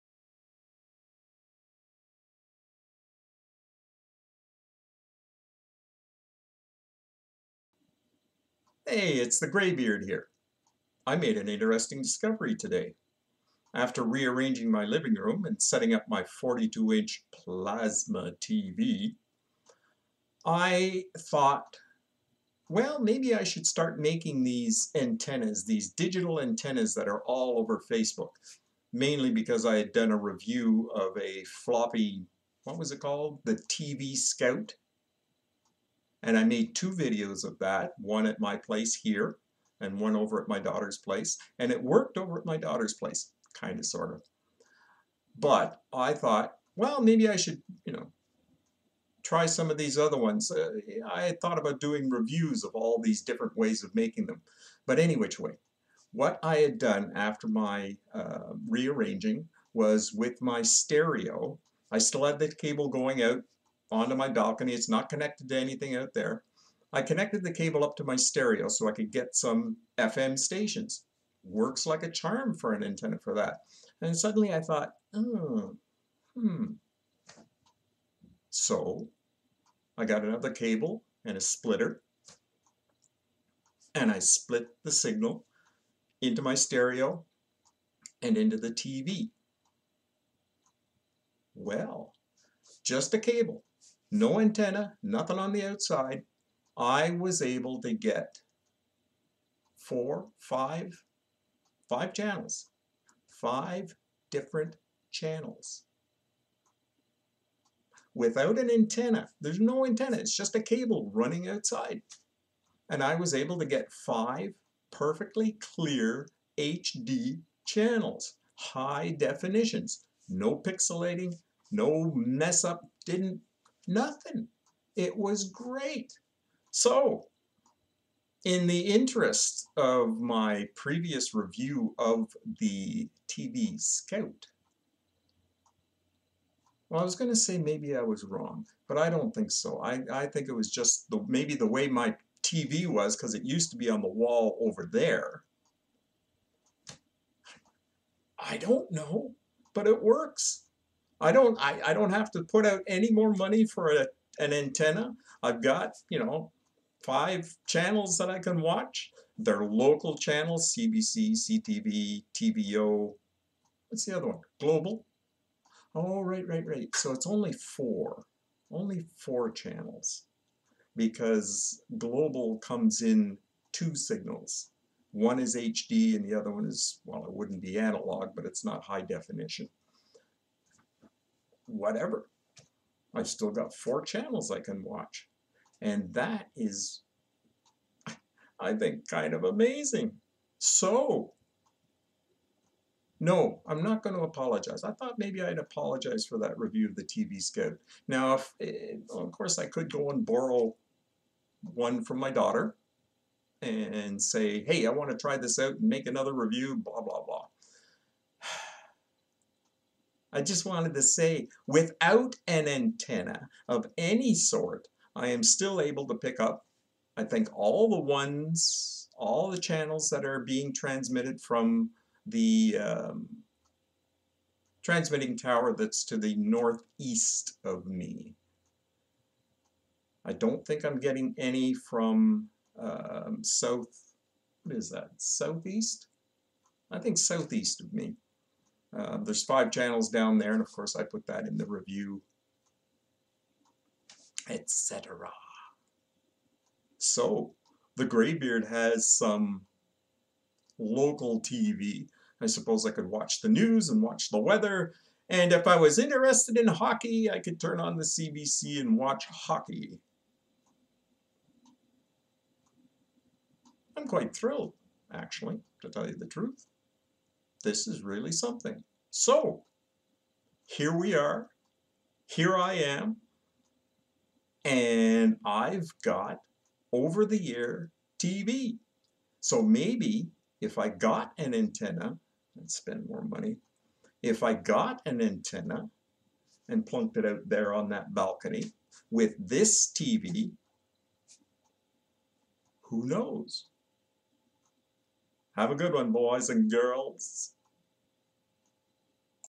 Over The Air (OTA) TV!
I did a review of an  OTA antenna I bought from a Facebook ad. It didn't work.
I just connected the cable going out on my balcony & got 4 (FOUR!) clear as a bell HD channels! Without any antenna except for the coax cable running about 5' on my balcony. Woo-Hoo!